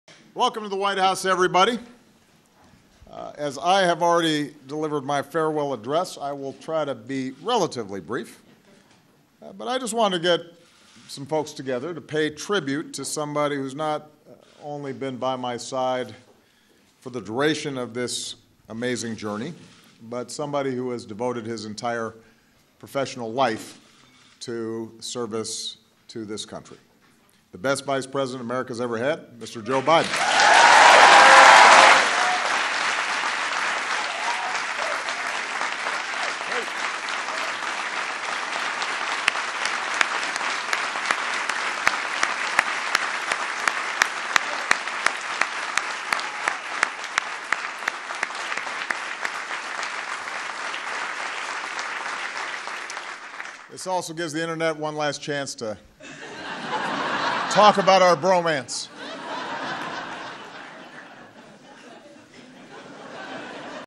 Obama jokes about 'bromance' with Biden
President Barack Obama pays tribute to Vice-President Joe Biden, awarding him the Presidential Medal of Freedom.